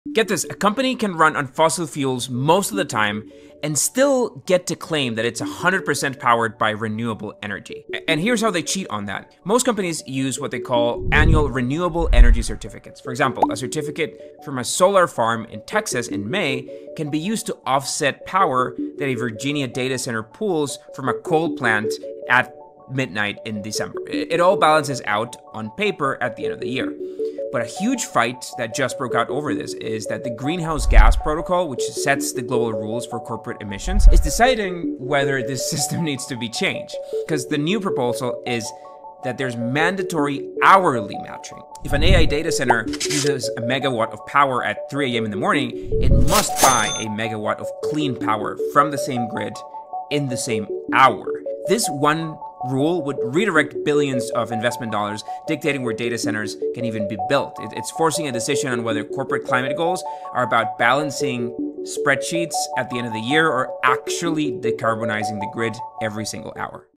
Your “100% renewable” company might be running on coal
How companies fake being “100% renewable.”

Here’s the trick: companies can run on fossil fuels all year and still claim they’re “fully powered by renewables.”

They buy annual renewable energy certificates (RECs), meaning a solar credit from May in Texas can “offset” coal power used in Virginia in December.

Now, the Greenhouse Gas Protocol wants to change that with hourly matching:
⚡ If a data center uses 1 MW at 3 a.m., it must buy 1 MW of clean power from the same grid, at the same hour.

This one rule could redirect billions in clean energy investment and finally decide whether “green energy” means real decarbonization or just creative accounting.